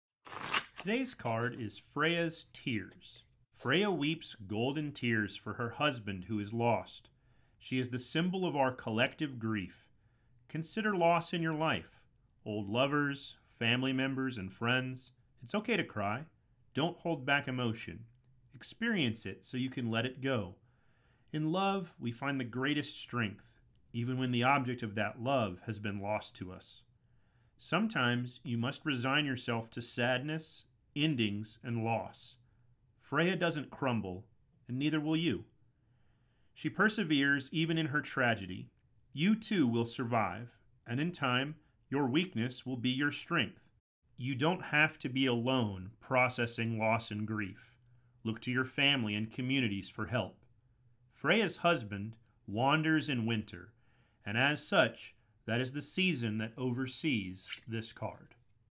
Viking Oracle Deck - Freya's Tears Card
Freya’s Tears
Loss is natural. We all experience loss. Freya weeps golden tears for her husband who is lost. She is the symbol of all of our collective grief. Consider the loss in your own life—old lovers, family members, and friends. It is okay to cry. This is not the time to hold back emotion, but a time to let go. It is in our love where we find the greatest strength, even when the object of that love has been lost to us.

Golden tears encourage us all to process our grief and remove the blocks we have put in place. Sometimes you must resign yourself to sadness, endings, and loss. Not all things can change, even if we want them too. However, Freya doesn’t crumble, and neither will you. She perseveres even in her tragedy. You too will survive, and in time your weakness will be your strength.

Freya’s husband wanders in winter, and as such that is the season that oversees this card. Winter is a time of loss and reflection. Be careful and remember you do not have to be alone in your loss. Look to your family and communities for help processing loss and grief.